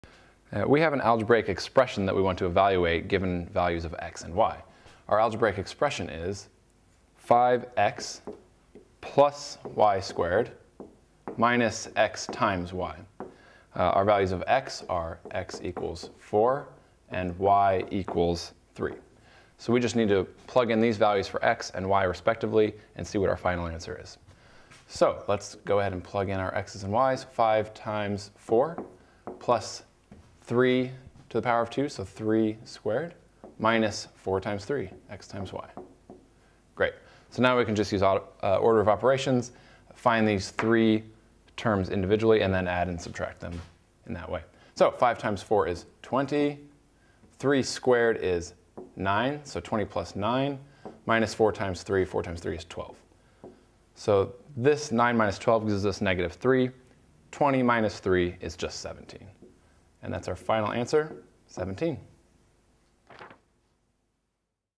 Evaluating Expressions with Given Values
Learn how to evaluate algebraic expressions using given values of x and y. Follow along as we plug in x=4 and y=3 into 5x+y^2-xy to find the final answer.